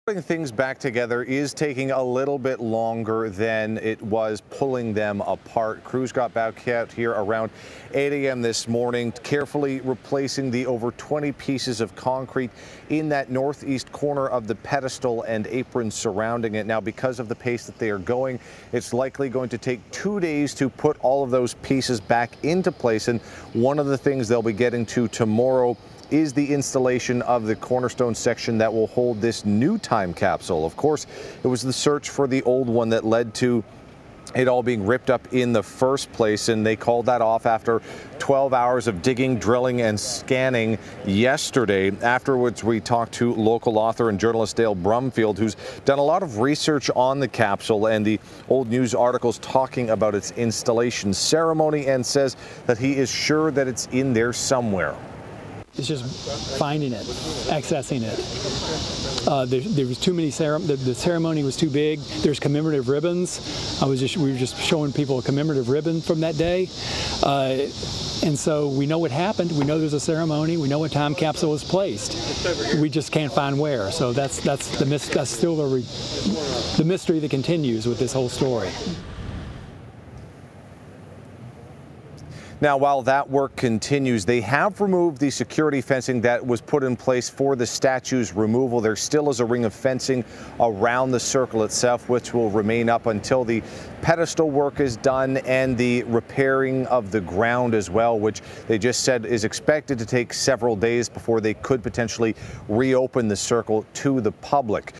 Lee statue base reassembled after failed time capsule search
Workers have returned to the Richmond site where a statue of Confederate Gen. Robert E. Lee stood for more than a century.